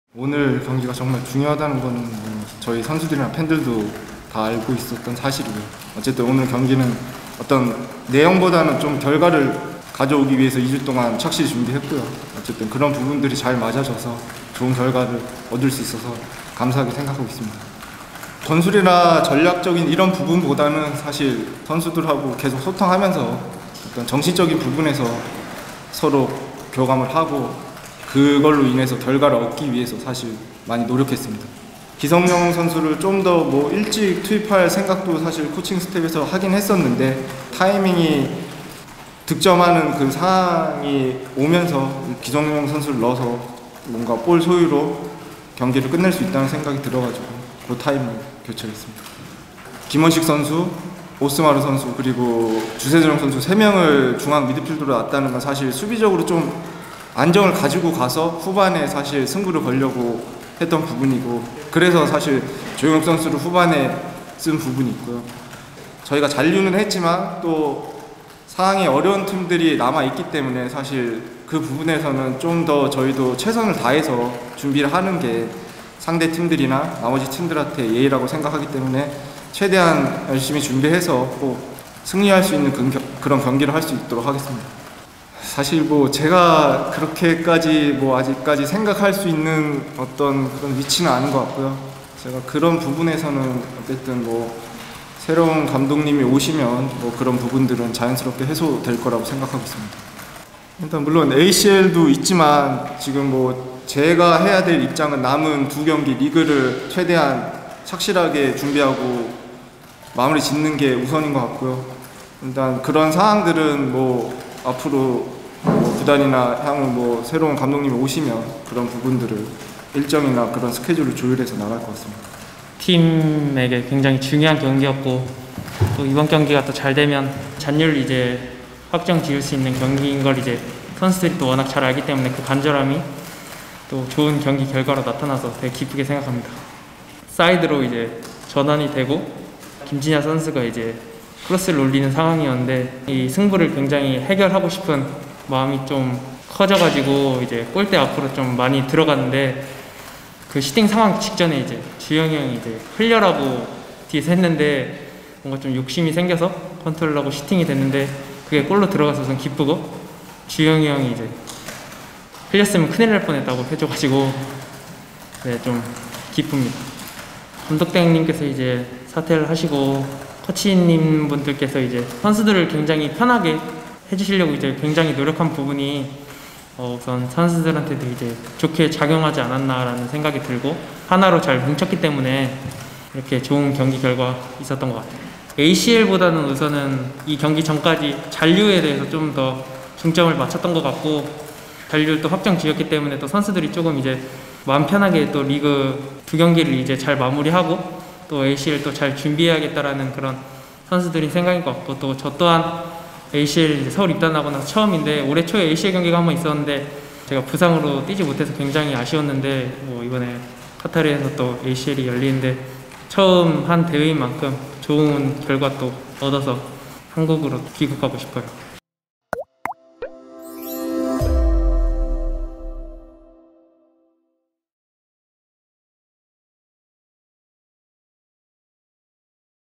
🔴⚫ 성남전 K리그1 R25 기자회견(20201017) ⚫🔴
"남은 경기에 집중해 좋은 결과 얻어 오겠다."
-조영욱-⠀